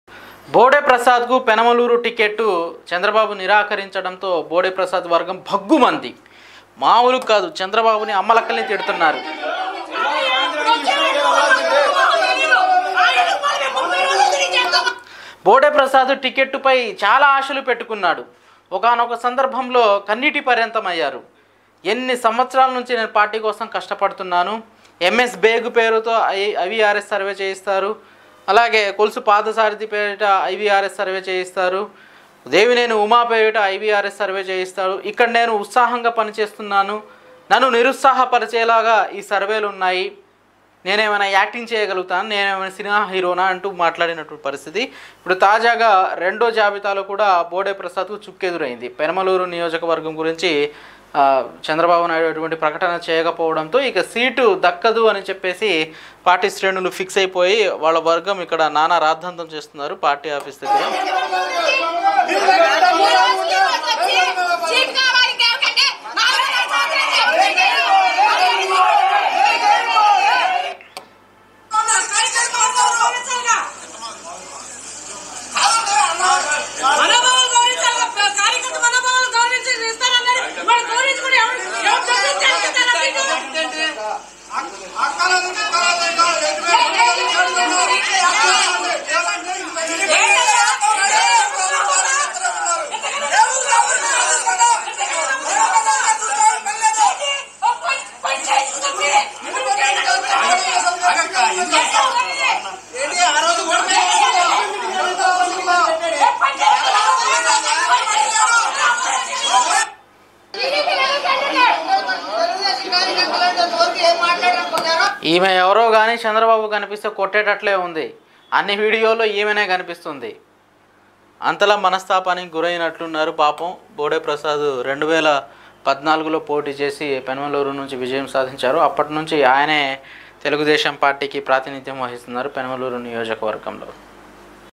Bode Prasad Activists Fire on Chandrababu : PDTV News
Bode Prasad Activists Fire on Chandrababu